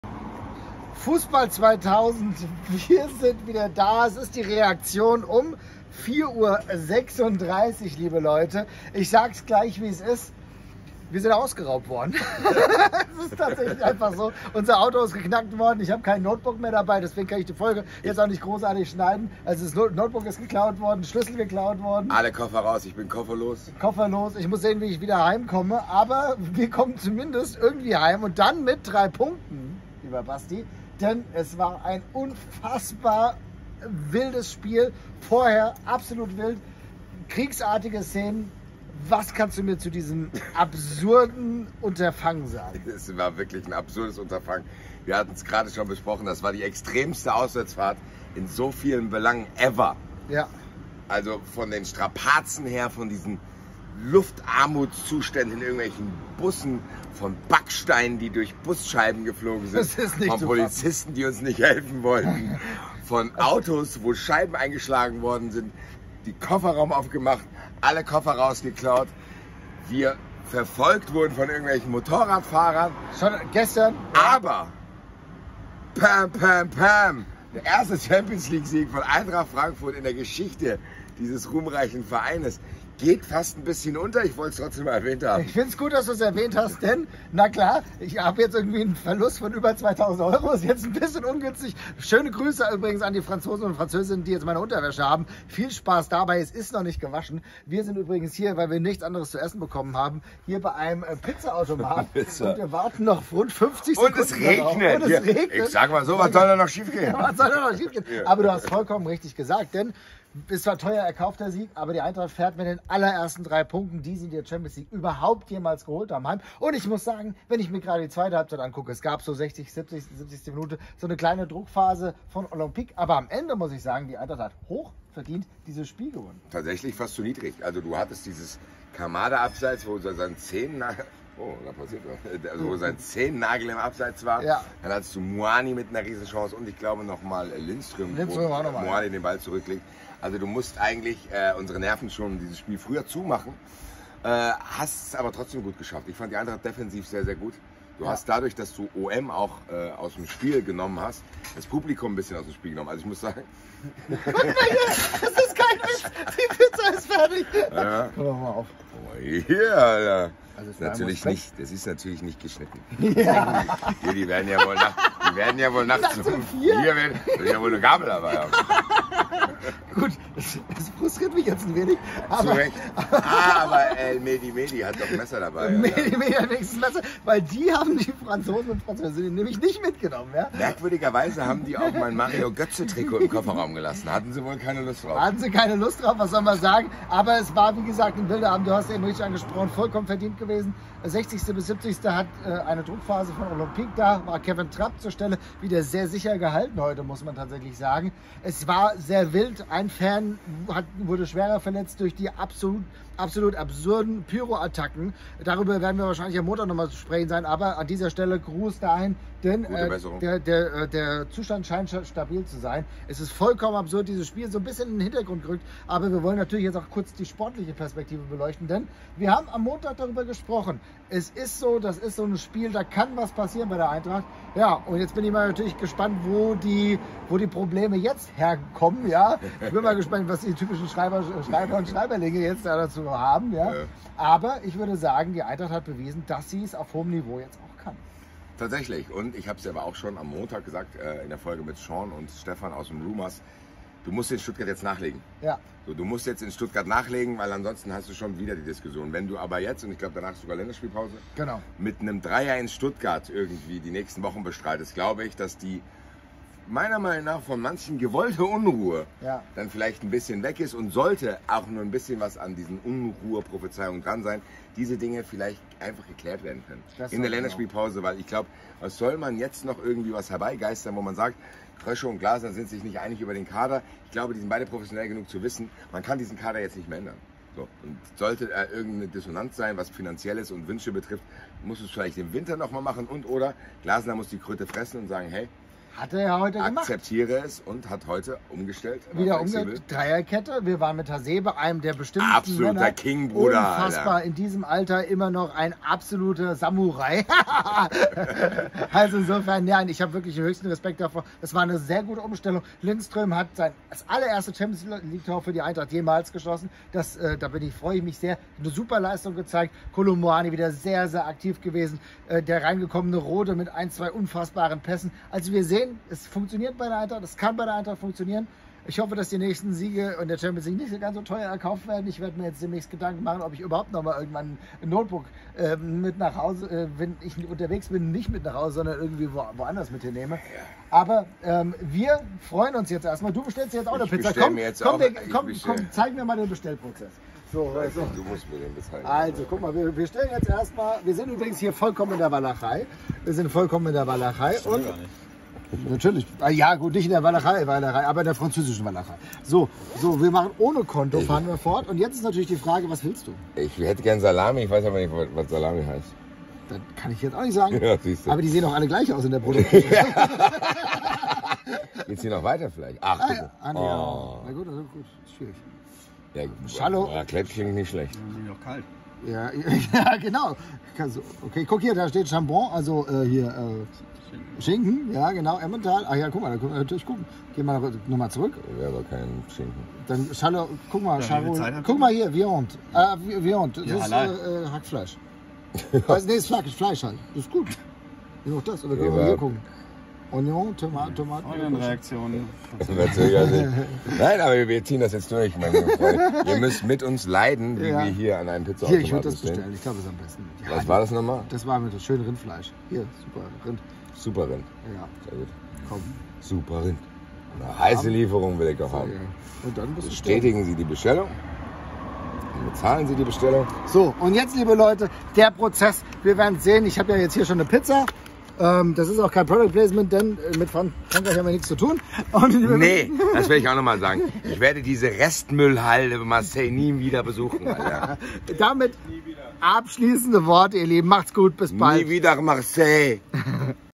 1:0 in Marseille! Chaos für viele Fans und ein historischer Sieg für Eintracht Frankfurt | Reaktion
Eintracht Frankfurt hat zum ersten Mal in der Vereinsgeschichte ein Champions-League-Spiel gewonnen. In Marseille endete das Duell gegen Olympique völlig verdient mit 1:0, doch der Sieg wird nicht das einzige sein, was langfristig in Erinnerung bleiben wird. Basti und Marvin berichten in ihrer ersten Reaktion von einer völlig heftigen Auswärtsfahrt!